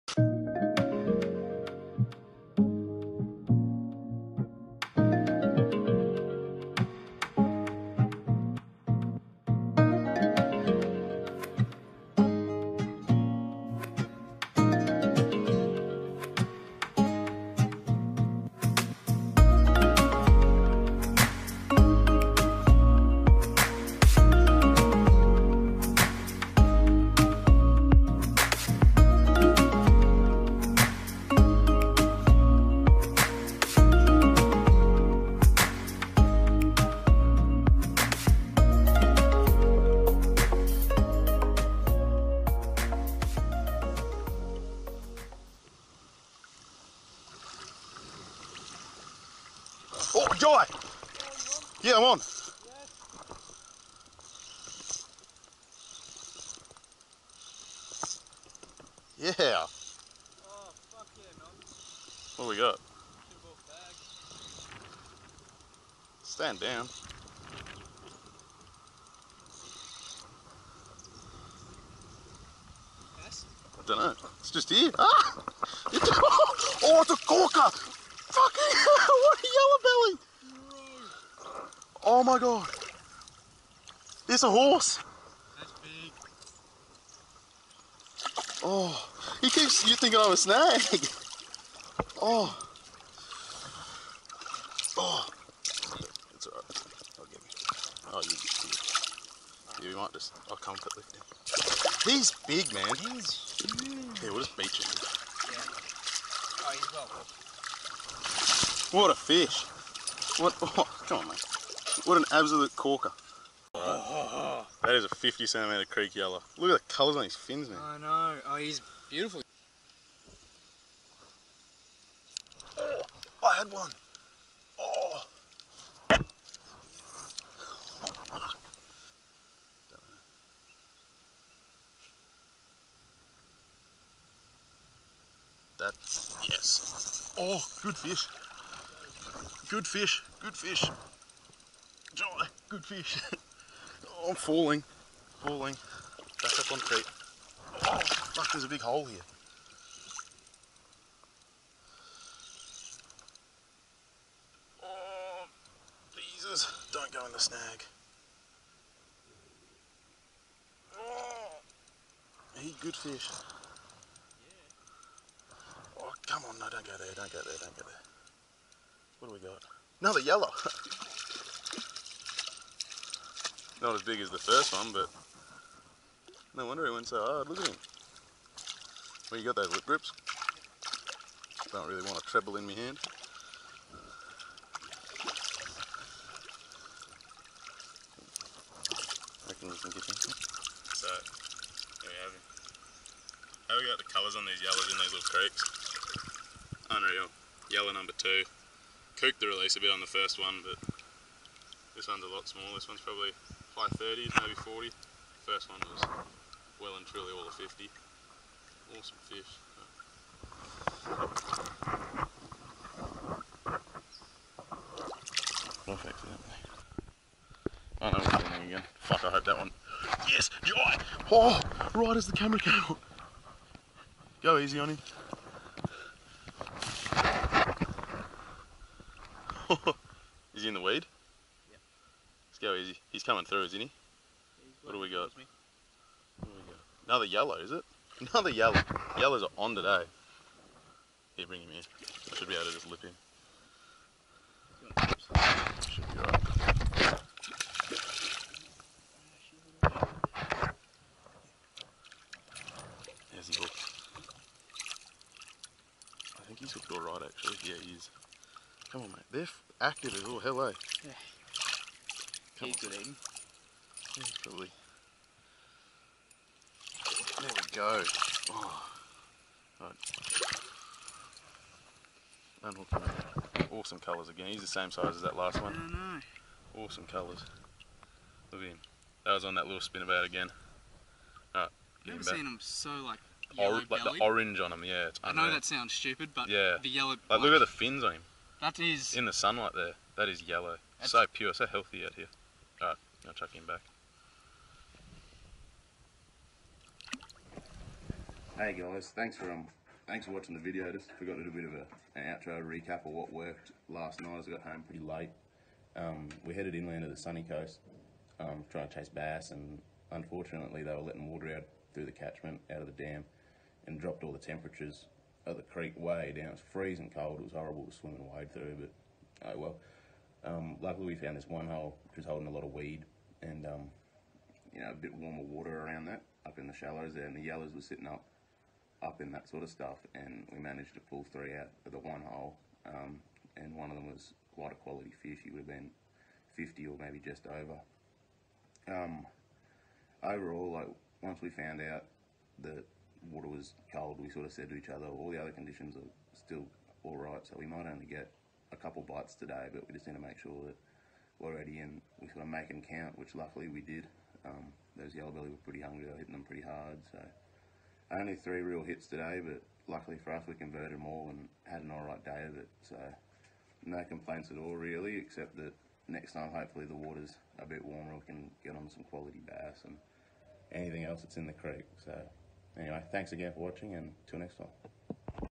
Sunshine Coast Freshwater Creek Fishing | 50cm Yellowbelly
Headed to the Sunshine Coast Hinterland to fish a small creek for Australian Bass. Unfortunately the creek was subject to dam water being released through the catchment which had dropped the water temp right down. Luckily for us in one hole a few yellas were found and seemed to be on the chew, hitting natural colours both in small hard bodies and spinnerbaits.

The gear used was 2-4kg spin and baitcast outfits with 10-15lb braid and 12-16lb leaders